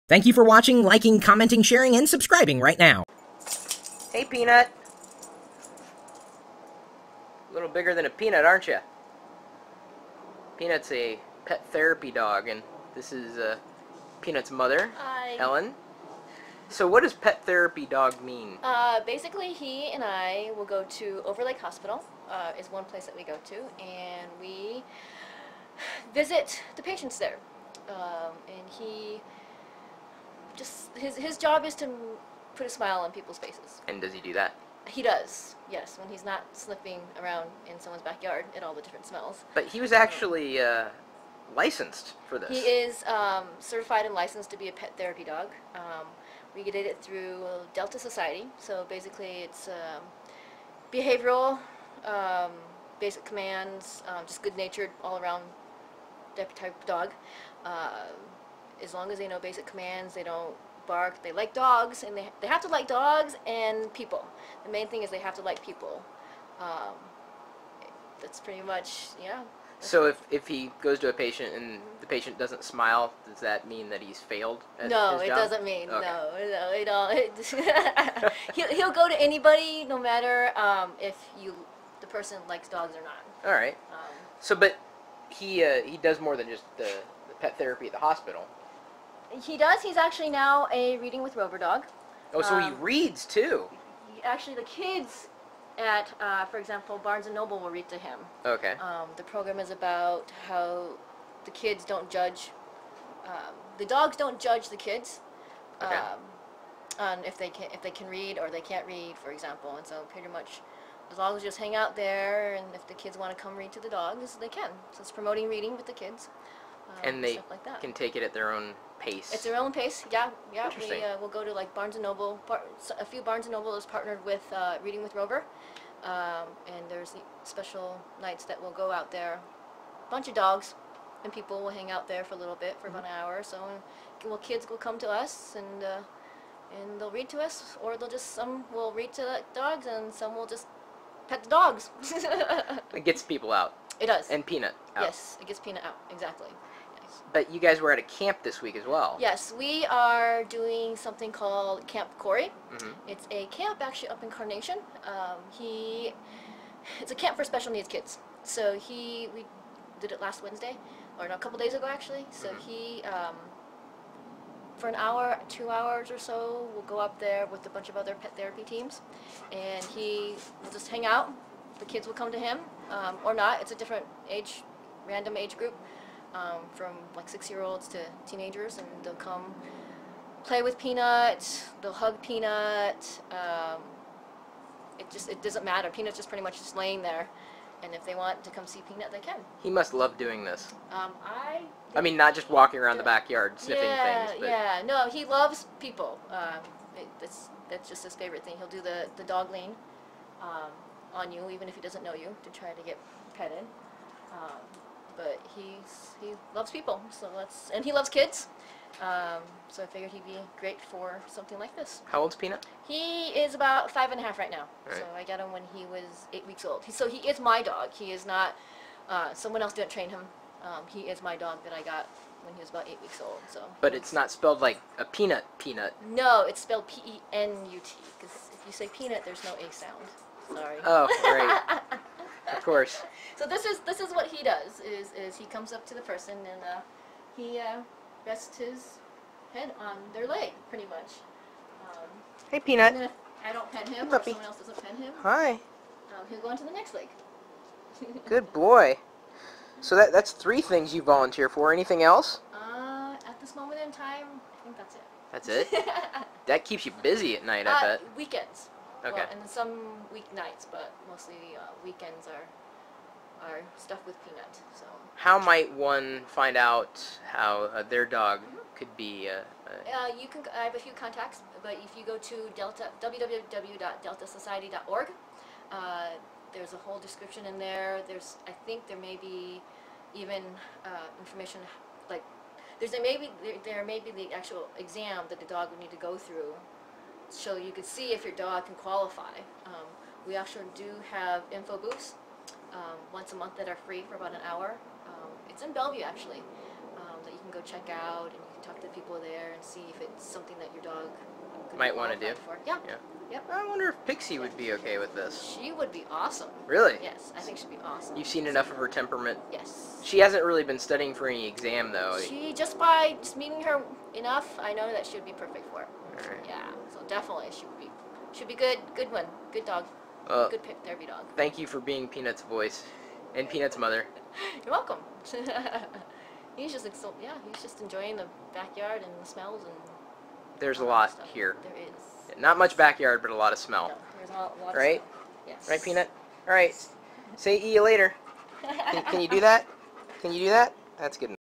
Pet Therapy Dog
/ - Meet Penut and Ellen. They love bringing joy to others. Woulld your pet be cut out for therapy (or should it be IN therapy)?